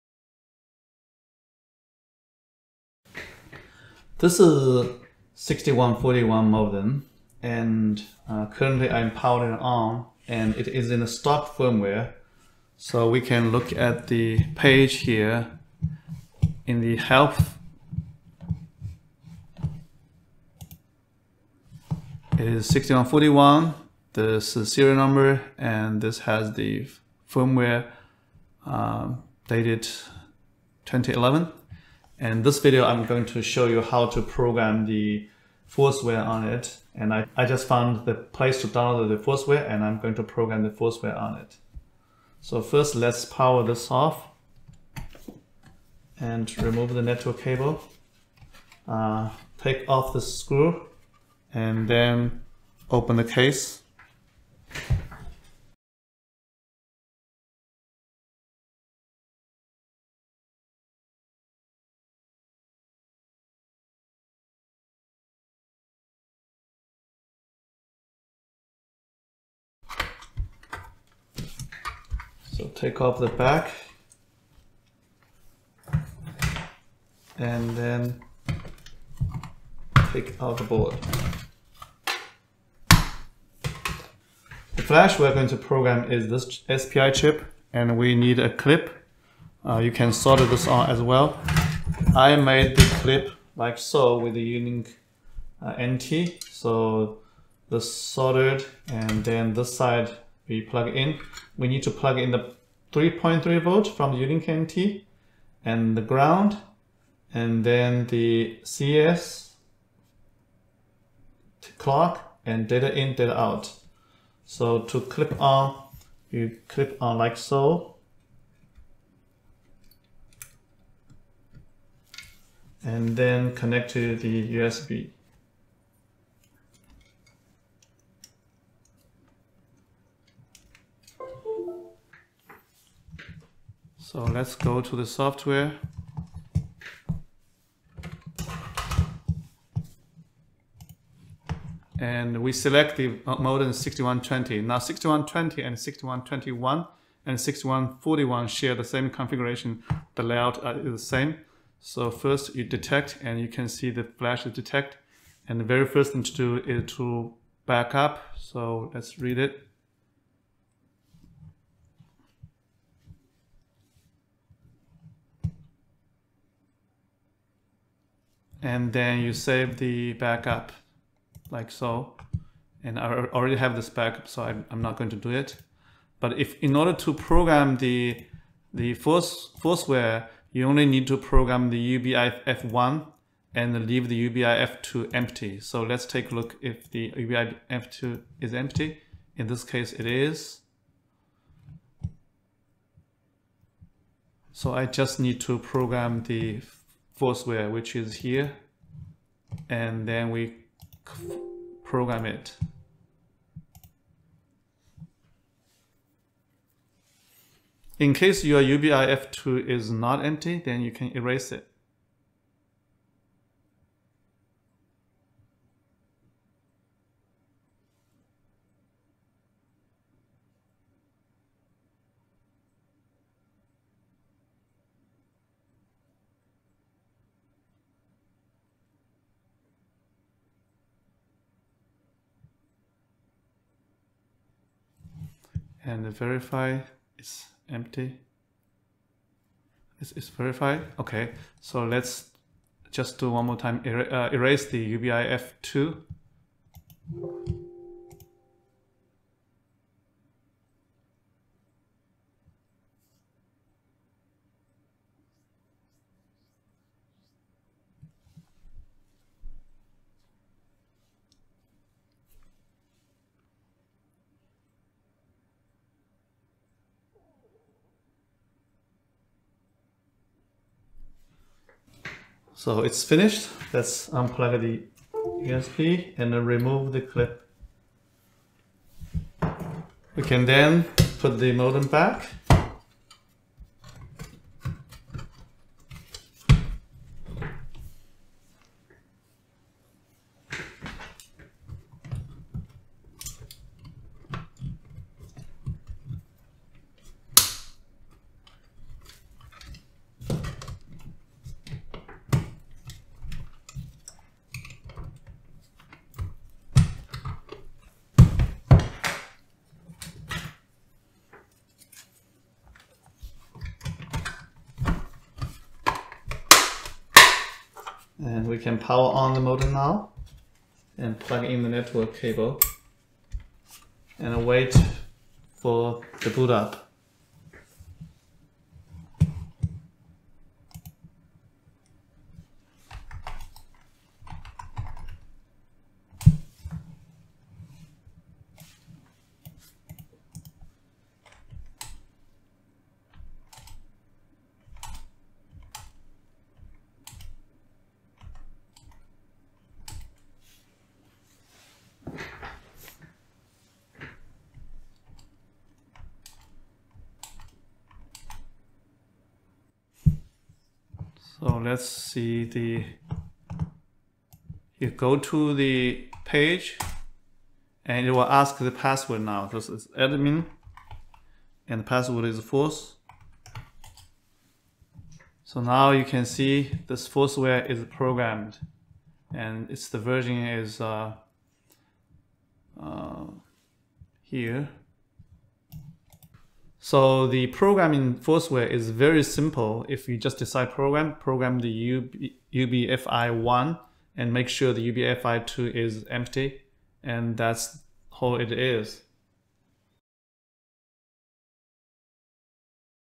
u-Link NT program Forceware on SB6141 modem.
Connect following pins.
3.3v, GND, CS,CLK(SCK),DO(SO),DI(SI).
Put the forceware on UBFI1, erase UBFI2.
Backup is optional but recommended.